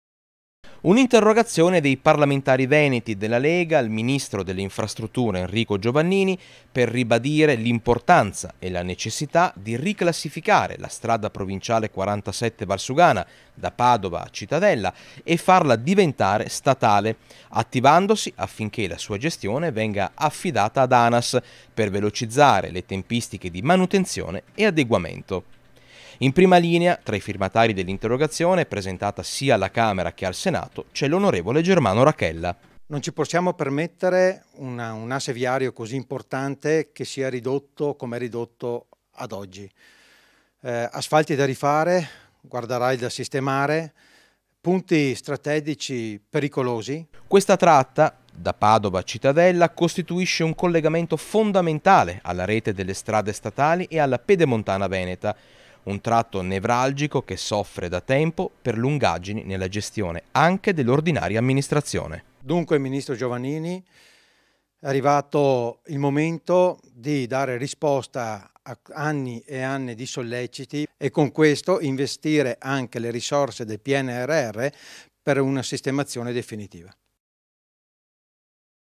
SP 47 VALSUGANA, ON. RACCHELLA: «DEVE DIVENTARE UNA STATALE, GESTITA DA ANAS» | 11/12/2021
TG Bassano del Grappa 11/12/2021 - CITTADELLA - Interrogazione dei parlamentari veneti della Lega al ministro Giovannini per trasformare la SP 47 Valsugana, nel tratto da Padova a Cittadella, in strada statale || Un’interrogazione dei parlamentari veneti della Lega al ministro delle Infrastrutture Enrico Giovannini per ribadire l’importanza e la necessità di riclassificare la strada provinciale 47 Valsugana, da Padova a Cittadella, farla diventa statale e attivarsi affinché la sua gestione venga affidata ad Anas per velocizzare le tempistiche di manutenzione e adeguamento. In prima linea fra i firmatari dell’interrogazione, presentata sia alla Camera che al Senato, c’è l’onorevole Germano Racchella. - Intervistati ON. GERMANO RACCHELLA (PARLAMENTARE) (Servizio di Alex Iuliano) - Segui Rete Veneta anche sul digitale terrestre! Visita il sito